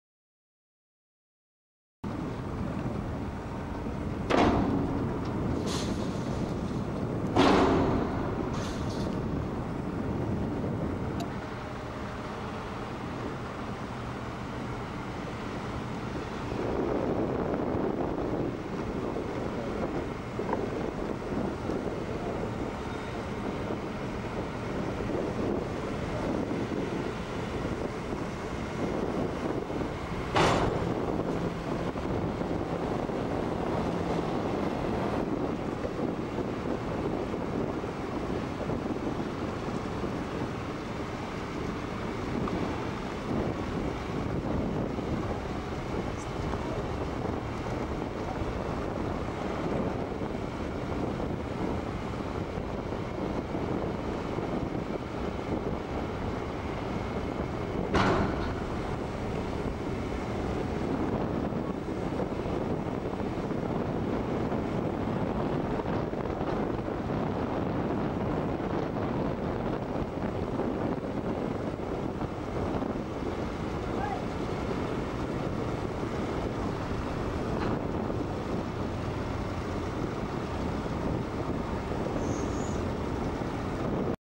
Loading shipping container with an on-ship crane
At major ports, shipping containers are typically loaded and unloaded by (stable) on-shore cranes.  However, in remote ports, an on-ship crane is needed. Very slight rocking of the ship at sea level can result in significant rocking at the top of the on-ship crane, resulting in a swinging of the container.
Worth considering when packing a shipping container.